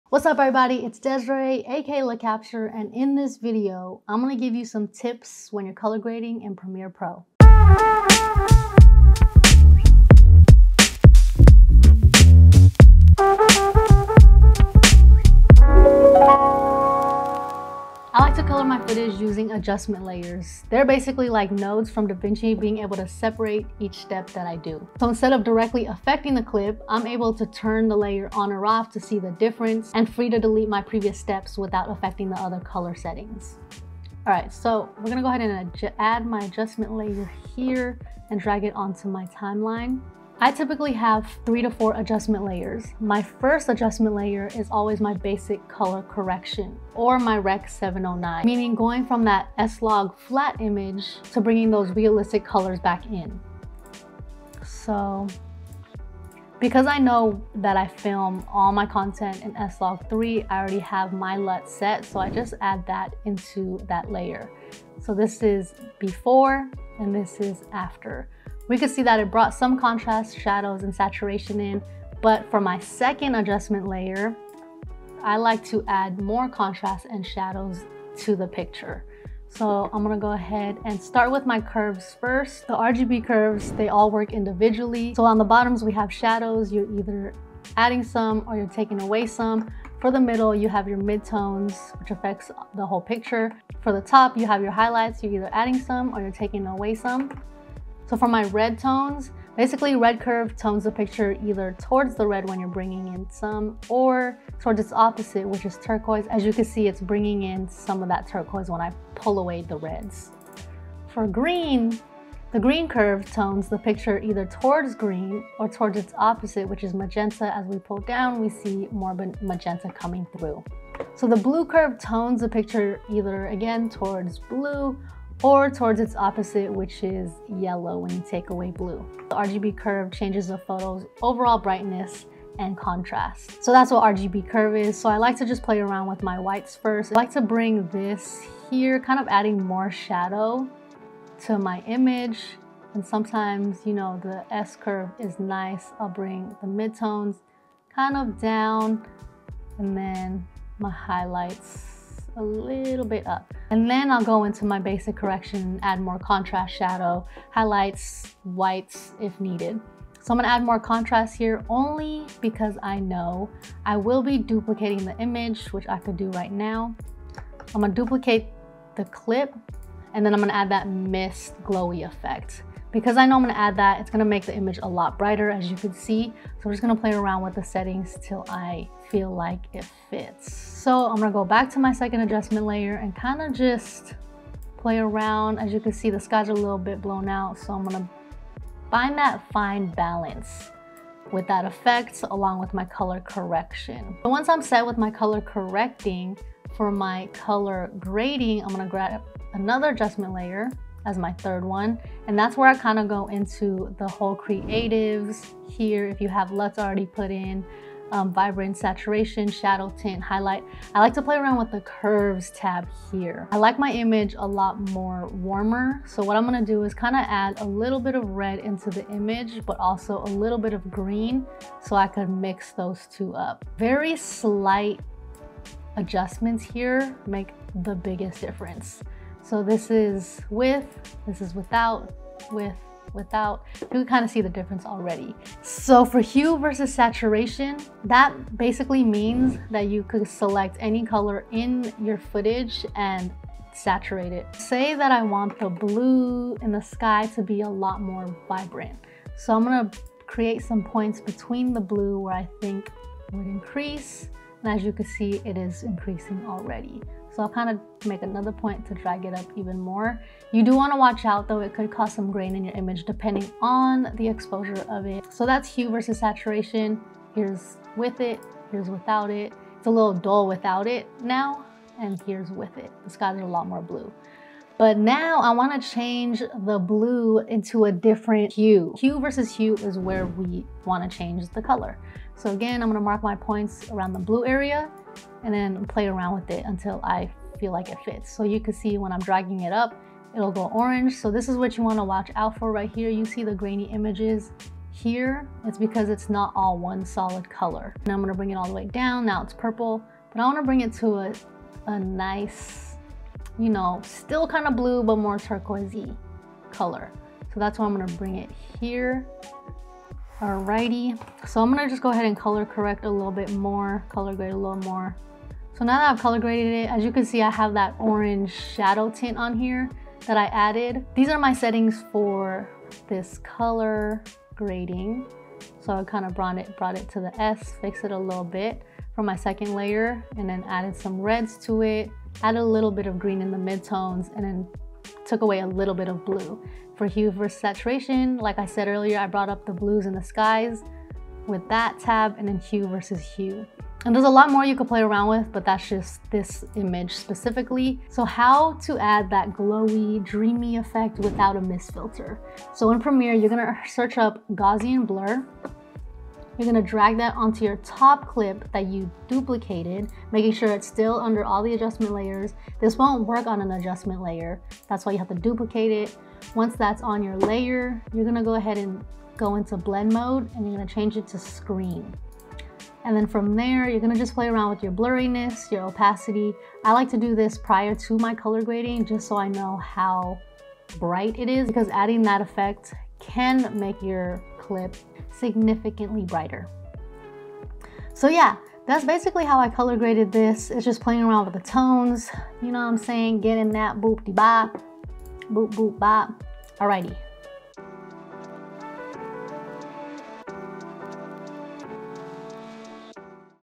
Color Grading Tips in Premiere Pro CC
Color correction in Premiere Pro is so important when editing videos. In this video, you'll learn some simple tips to remember to get your color correcting and grading to the next level.

If you you're looking to make the look of your films standout and get your career to the next level but struggle with the color grading aspect... start with these simple tips and practice as you go! See what works for you and doesn't and you'll start to understand it more and more each time.

I've been where you have been, so I know that the Lumetri scopes, settings and all can look intimidating - but you can do it! If you're just beginning, let my channel be a guide to help you feel confident as a filmmaker and editor!

ALL THE GEAR I USE: